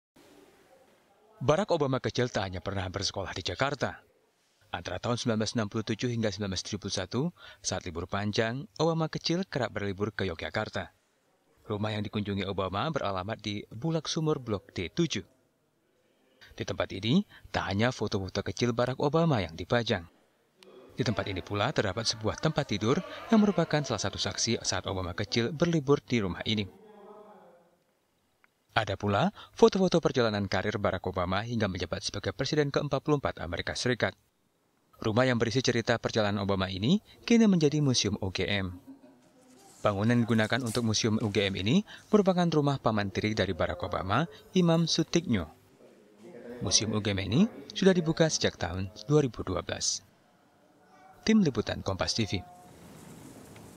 Mengenang Masa Kecil Obama di Museum UGM
Barack Obama kecil tak hanya pernah bersekolah di Jakarta. Antara tahun 1967 hingga 1971, saat libur panjang, Obama kecil kerap berlibur ke Yogyakarta. Rumah yang dikunjungi Obama beralamat di Bulaksumur Blok D7. Di tempat ini tak hanya sekadar foto-foto kecil Barack Obama yang dipajang.